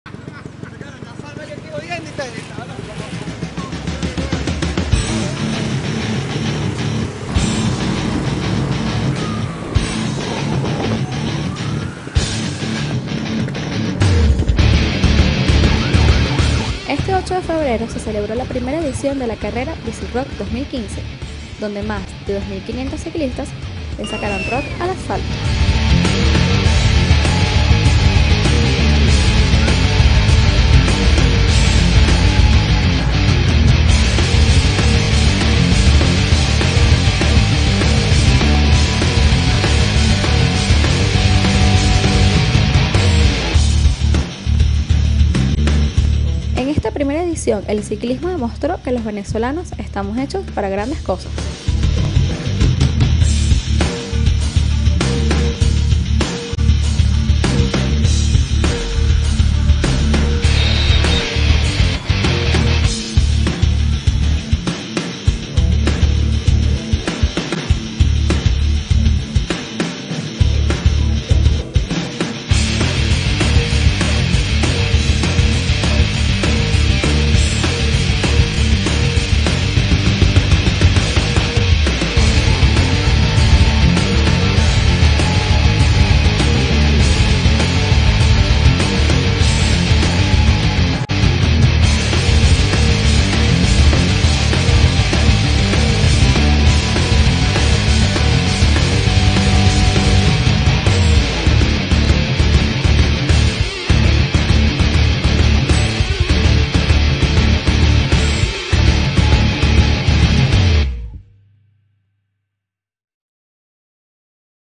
BiciRock 2015
Este 8 de febrero se realizó la primera edición del Gatorade BiciRock 2015, donde participaron más de 2500 ciclistas.